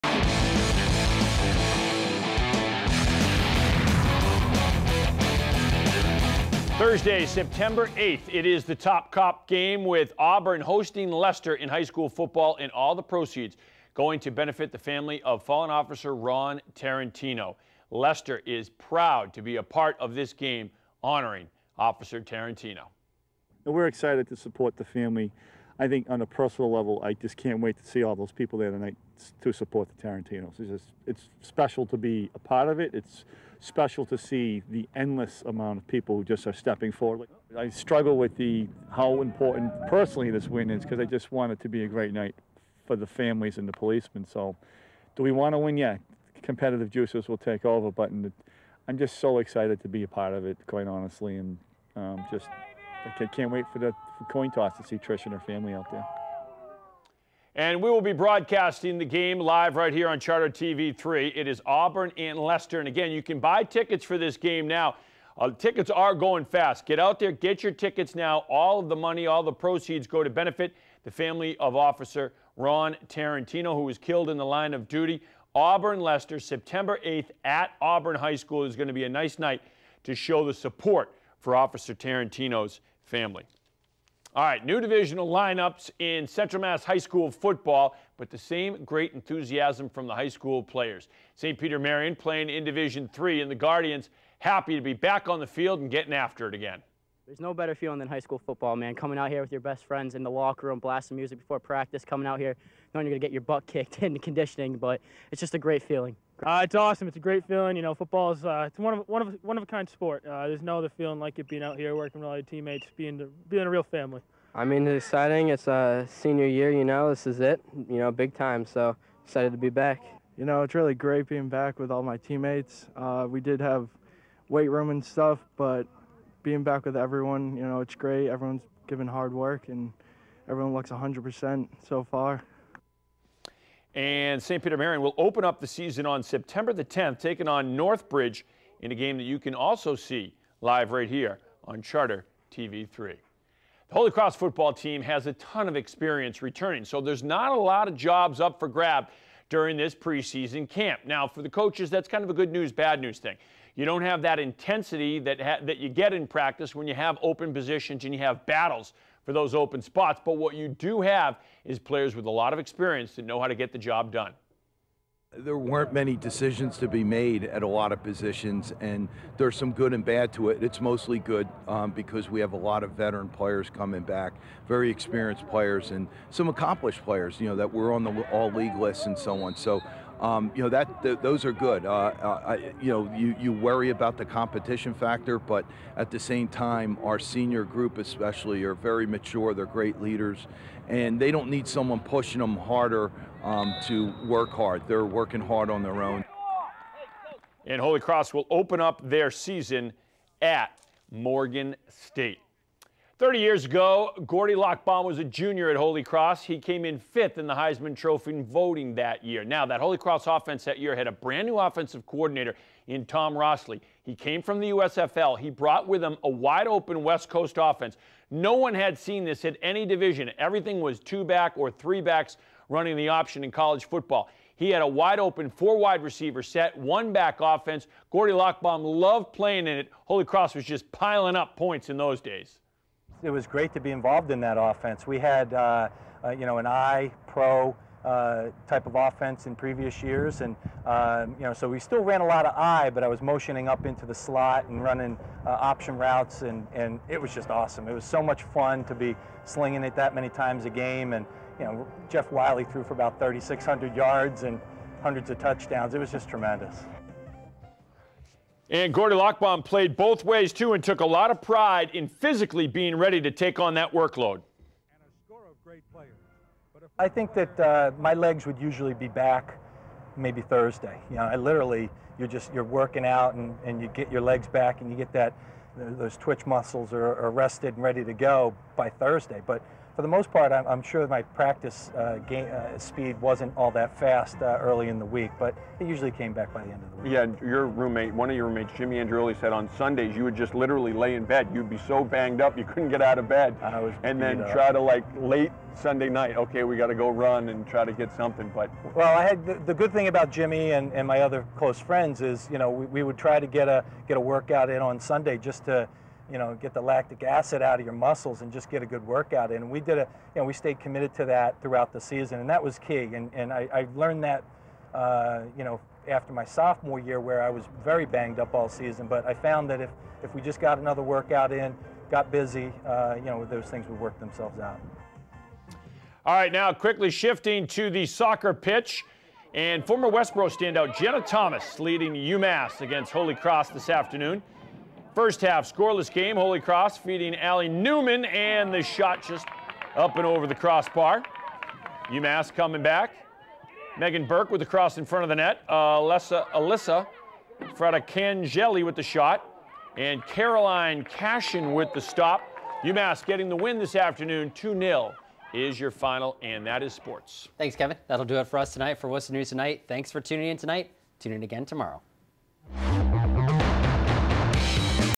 WNT Sports - August 22nd, 2016
Kevin Shea as a look at Leicester, SPM and Holy Cross Football, Talks to Gordie Lockbaum and has highlights from Holy Cross-UMass Womens Soccer.